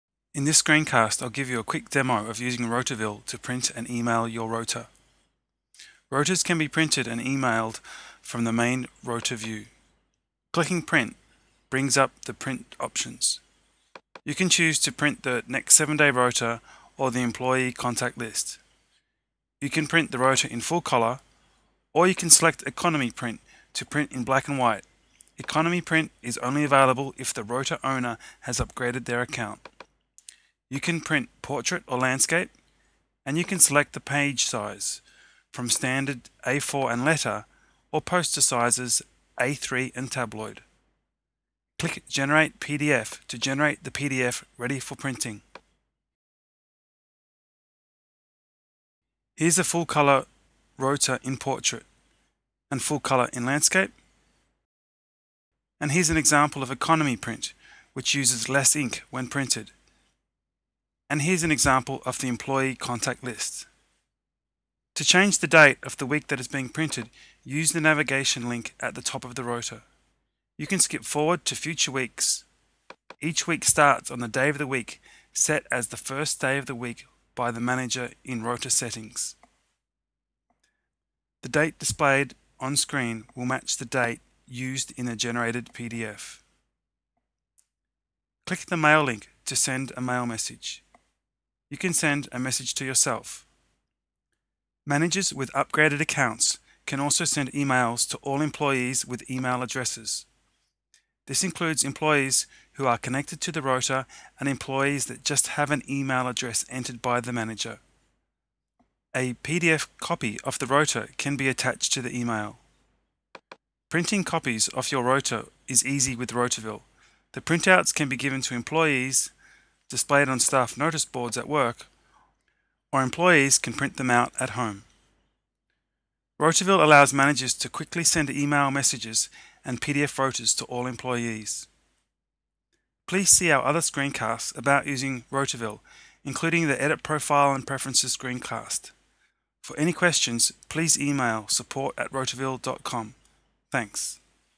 Printing and Emailing the Weekly Rota - Rotaville Shift Scheduling Software
A demostration showing them how to use Rotaville for printing and emailing PDF copies of the rota.